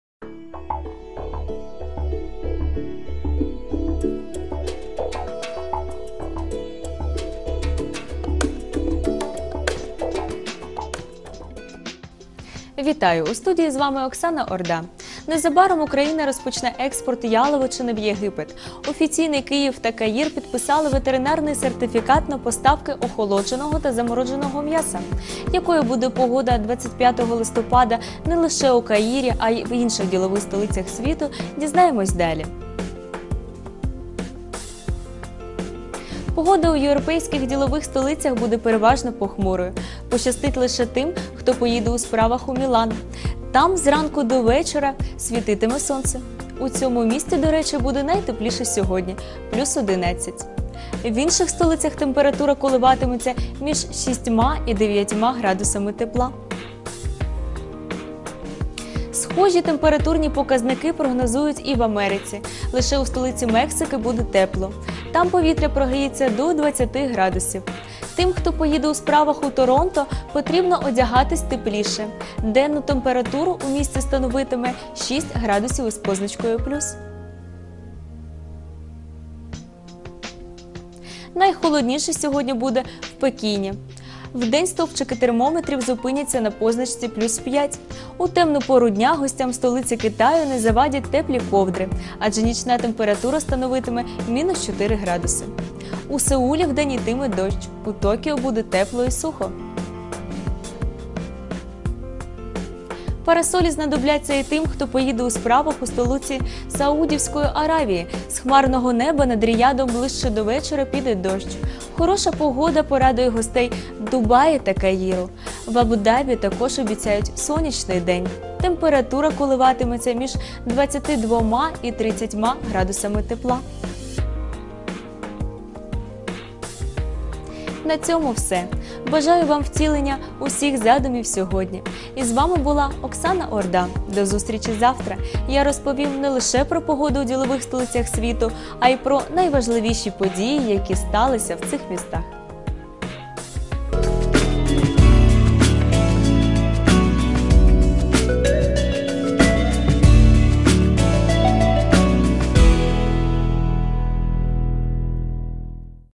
Погода. Деловые столицы. 26 ноября
Прогноз погоды в крупнейших бизнес-столицах мира на субботу, 26 ноября 2016 года.